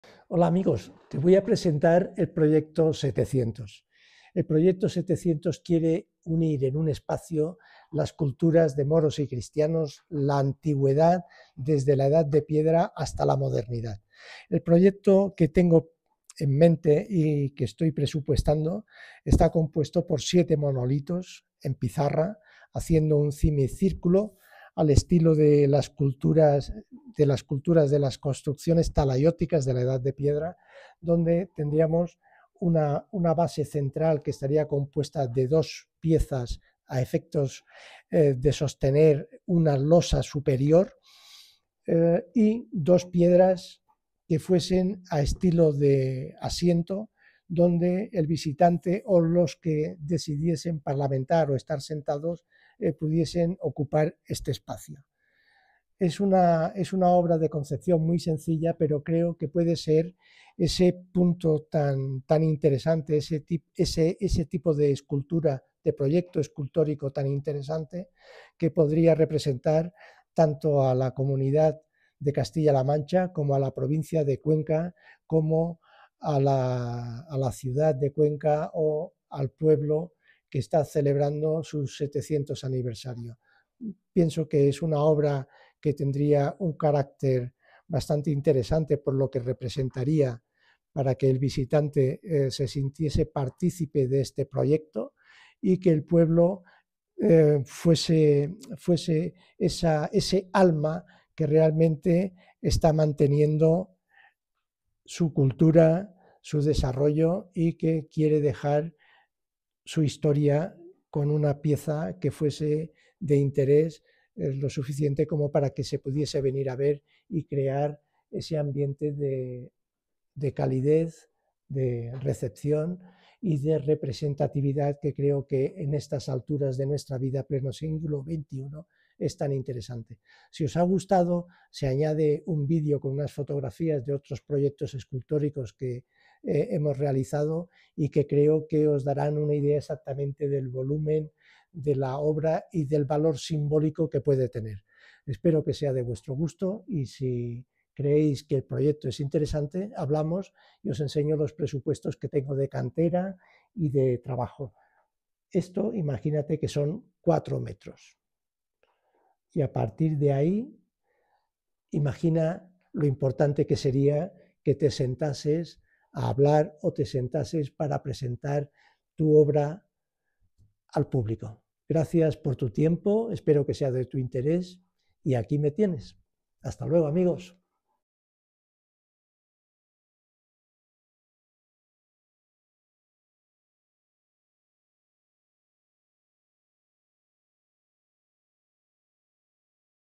Escultura Urbana Presentación Proyecto Escultórico 700 Aniversario
Importancia de La Escultura en el Espacio Urbano: Presentación del boceto para una escultura urbana a base de monolitos de pizarra y piedra sobre una base de pizarra negra con una pieza de estilo talayótico representando una "tabla superior sujeta por dos monolitos"* representando las dos grandes culturas de nuestra sociedad manchega, por una parte la cristiana y por otra para la musulmana y dos asientos de piedra en donde sentarse a hablar, meditar o cerrar tratos y acuerdos. *Pieza central se puede realizar en piedra o en Vidrio Laminado Templado, obra que ya se ha realizado hace años en La Plaza Redonda de Valencia a través de nuestra Sociedad

La escultura en el Espacio Urbano, se logra mediante una contextualización y comunicación muy íntima con el entorno que la enmarca, donde se integra, actúa y transforma la esencia plástico-cultural de los habitantes. El desarrollo del proyecto escultórico se vuelve relevante a los ojos del espectador a medida que es coherente con la conceptualización, el enfoque, la percepción del escultor sobre la realidad del entorno en donde se debe ubicar, y el tipo de representación o modelo que se ha diseñado para el espacio público que se a previsto.

En el proceso de diseño y esbozo previo a una maqueta se han utilizado métodos teóricos y empíricos, que permitan conseguir un objetivo de representación simbólica al tiempo que moderno e integrador asumido desde el dominio de la arquitectura, el diseño y su vertiente artística  y de su impacto económico en el entorno urbano en el que se debería ubicar.

Del enfoque biocéntrico plasmado en la obra escultórica del resultado del diseño y del esbozo mostrado, se puede destacar: Que la escultura no puede ser pura intuición, en la concepción que nos identifica, la acción como parte operativa en la realidad concreta, debe ir unida a la imaginación, la creatividad, la imagen proyectiva, denotando simbiosis entre el manejo conceptual y el conocimiento de la técnica que se aplica, que dota al escultor de sensibilidad para materializar lo abstracto y entender la constante transformación de la realidad. .

En la obra presentada a valoración para éste proyecto interpreto que existe una oposición entre arte y técnica que el proyecto escultórico resuelve en cierta medida, al mostrar a la sociedad en su entorno histórico que el mismo arte es una técnica en los dos niveles de las actividades operatorias y figurativas.

Se comparte que la escultura no puede ser pura intuición; en la concepción que nos identifica, la acción como parte operativa en la realidad concreta, debe ir unida a la imaginación, la creatividad, la imagen proyectiva, denotando simbiosis entre el manejo conceptual y el conocimiento de la técnica que se aplica, mejorada cada vez que se va logrando madurez artística y perfeccionamiento de habilidades y destrezas; por otro lado, la actividad creativa del artista plástico, refleja la concepción que tiene del contexto en el que opera, esto es, las condiciones objetivas y subjetivas tienen una armoniosa conjugación dialéctica, esos estímulos emocionales le permiten codificar y plasmar un lenguaje plástico único, muy identificativo, que responde al conocimiento de aquella realidad observada.